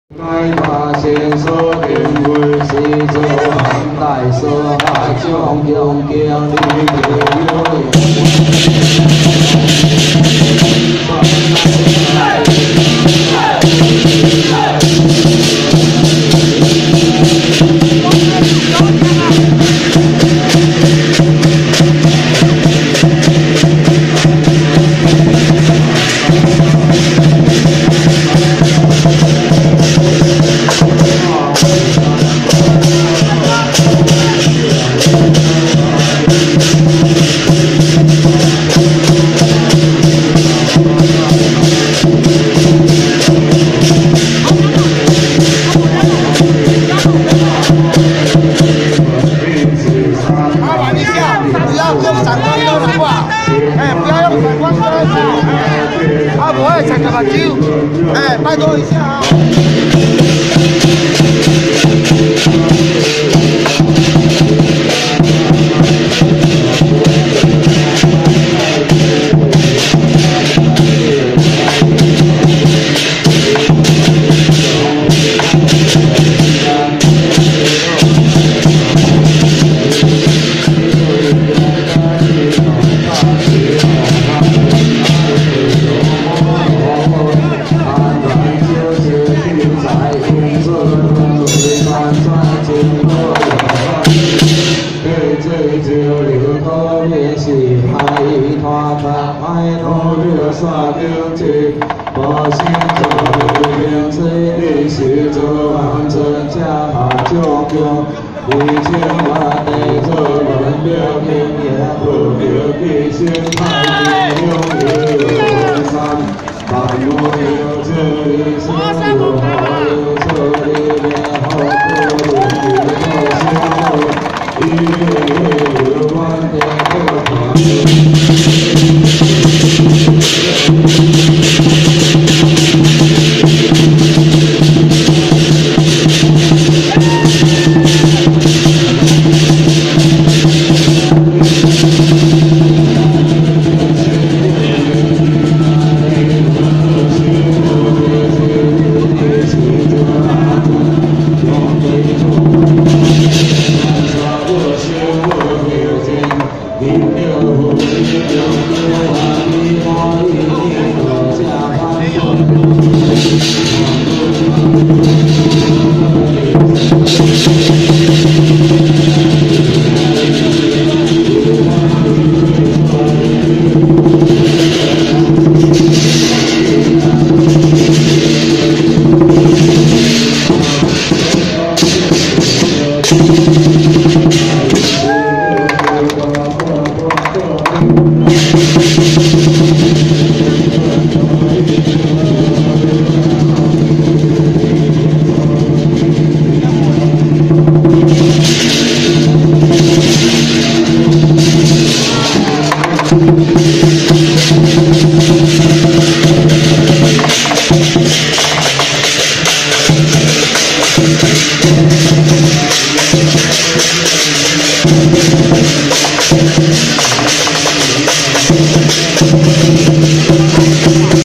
2019松山慈惠堂 臺北母娘文化季 保民遶境嘉年-臺中晟德館醒獅跳樁
台視新聞公司前拍攝